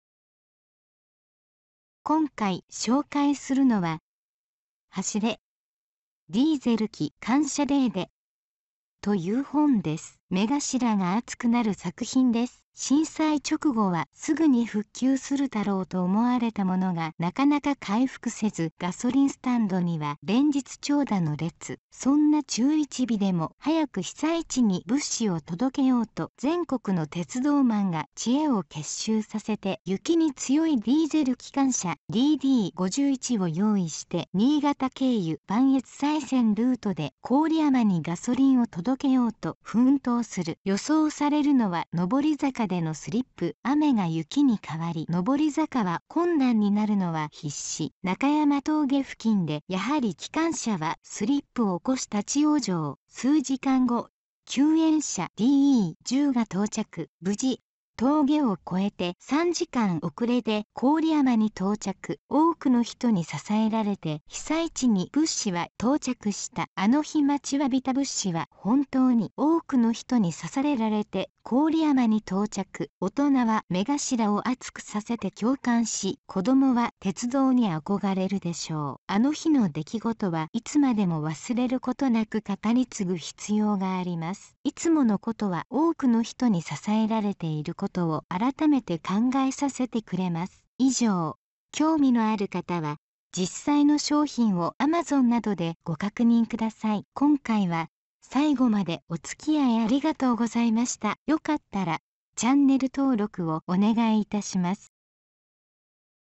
【紹介】はしれ ディーゼルきかんしゃデーデ 絵本・こどものひろば （すとう あさえ,鈴木 まもる）
今回紹介するのは、　はしれ ディーゼルきかんしゃデーデ　という本です。

目頭が熱くなる作品です。

震災直後はすぐに復旧するだろうと思われたものがなかなか回復せずガソリンスタンドには連日長蛇の列。
そんな中１日でも早く被災地に物資を届けようと全国の鉄道マンが知恵を結集させて雪に強いディーゼル機関車
DD51を用意して新潟経由磐越西線ルートで郡山にガソリンを届けようと奮闘する。予想されるのは登坂でのスリップ。
雨が雪に変わり登坂は困難になるのは必至。中山峠付近でやはり機関車はスリップを起こし立ち往生。
数時間後、救援車DE10が到着。無事、峠を越えて３時間遅れで郡山に到着。
多くの人に支えられて被災地に物資は到着した。
あの日待ちわびた物資は本当に多くの人にさされられて郡山に到着。
大人は目頭を熱くさせて共感し子どもは鉄道にあこがれるでしょう。
あの日の出来事はいつまでも忘れることなく語り継ぐ必要があります。
いつものことは多くの人に支えられていることを改めて考えさせてくれます。